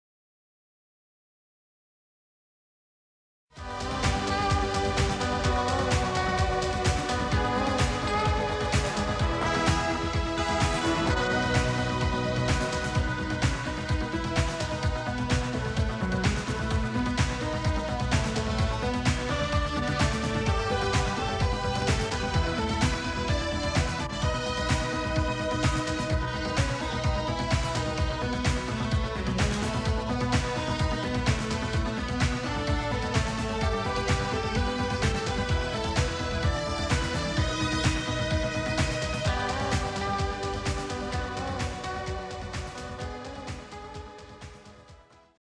ken drone filming at hub
Drone footage at the Steven Burke Hub, Barrowford.